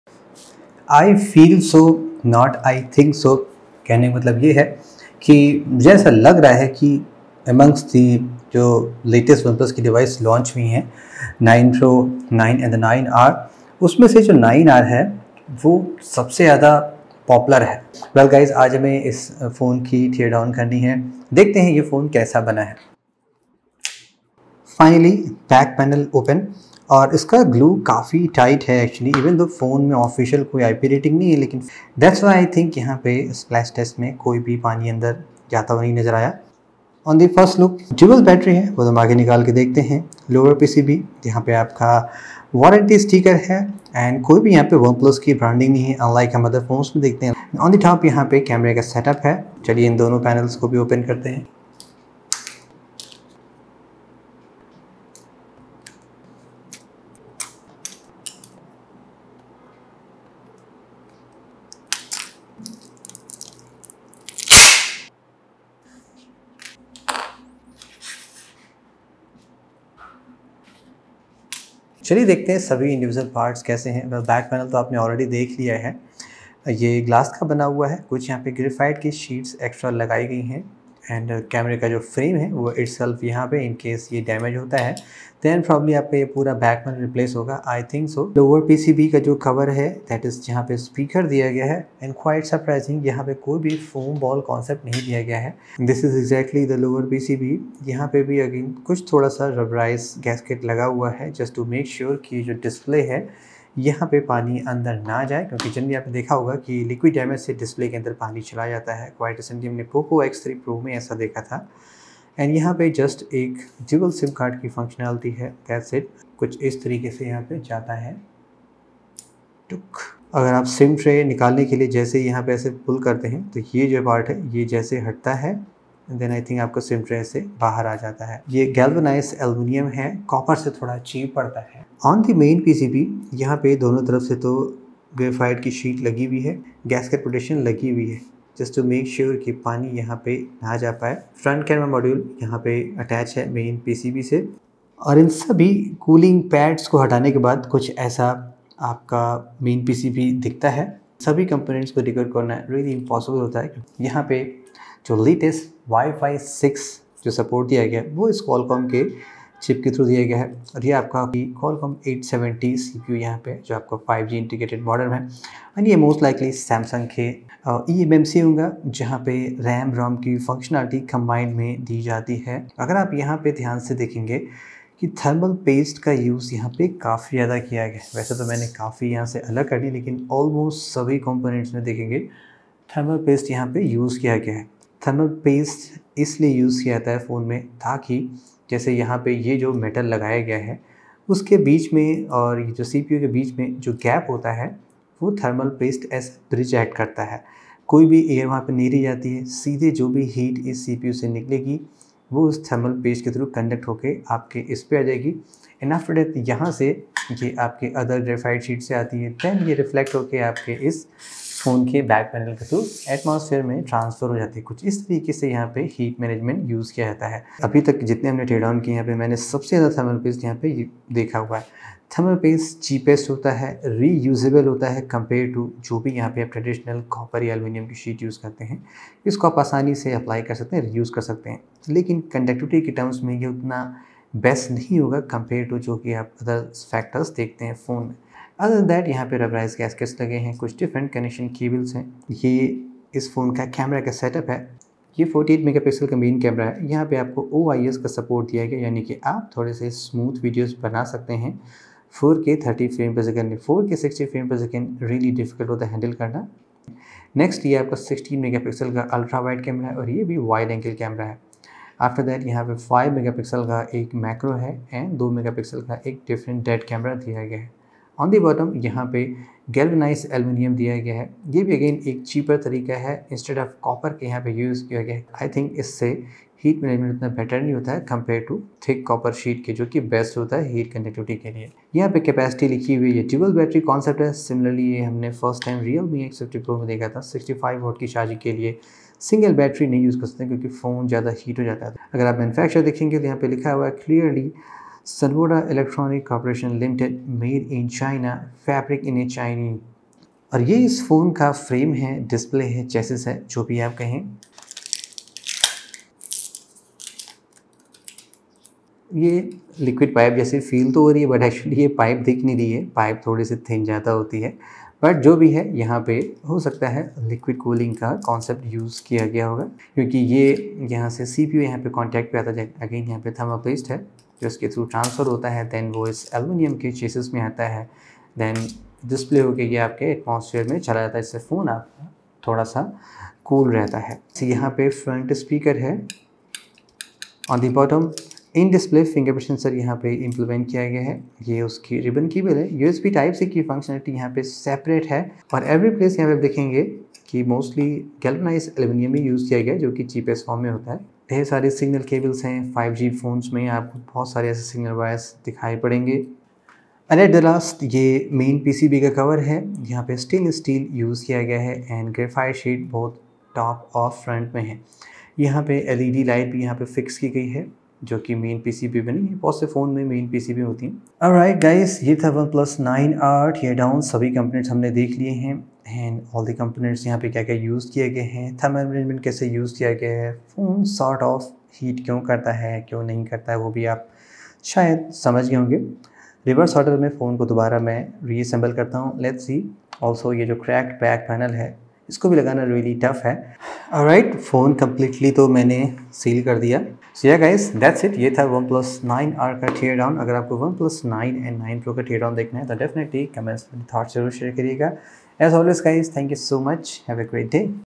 OnePlus 9R Teardown - Lot's of Thermal Paste , Still Heating Issue ?
In today's what inside episode we will checkout the OnePlus 9R.  see what are the components inside it and why this phone is heating.
See our Host Saurabh on his social network,